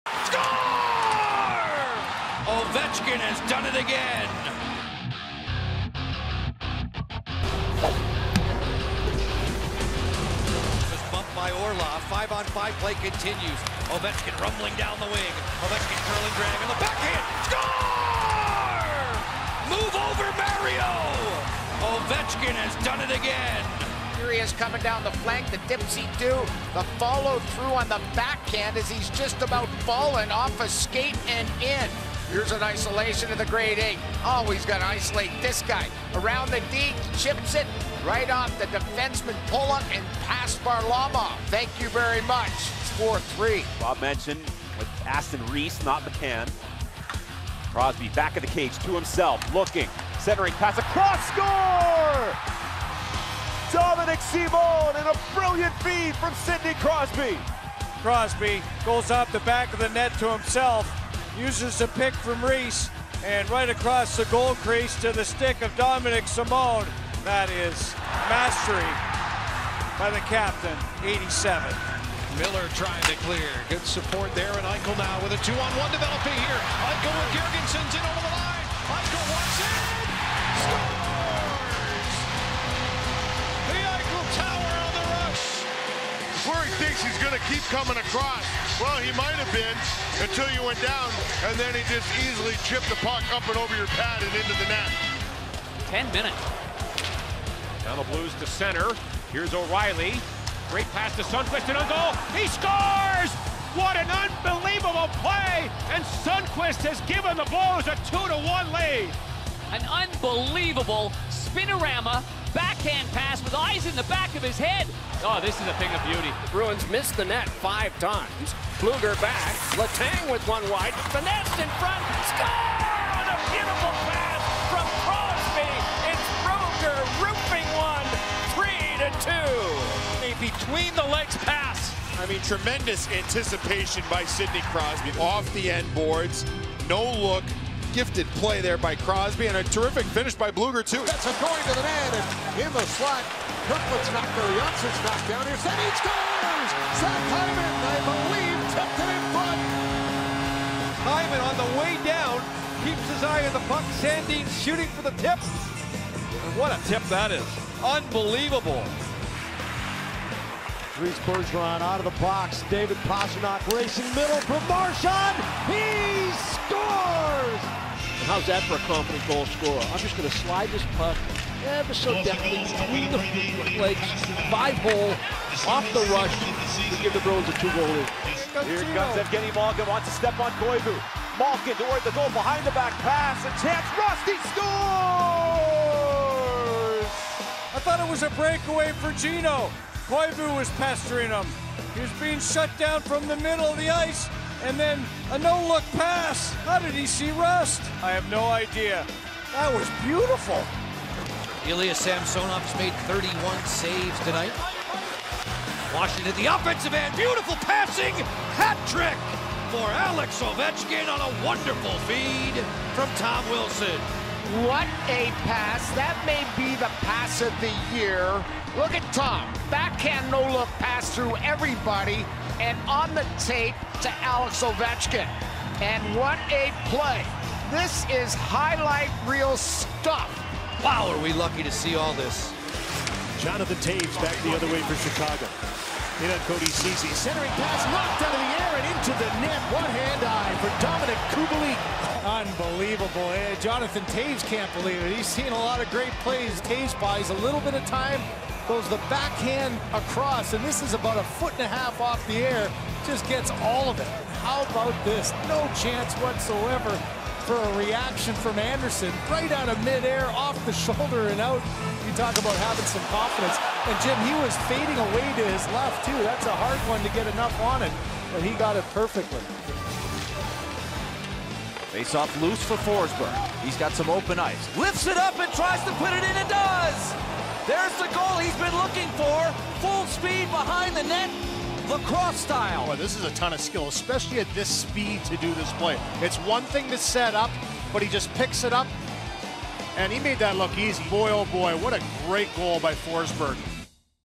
NHL Goals Of The Week: Dominik Kubalik Bats One In
All the top goals from the week that was in the NHL, including Sidney Crosby's series of amazing passes, and Dominik Kubalik batting in the unreal cross ice pass from Jonathan Toews.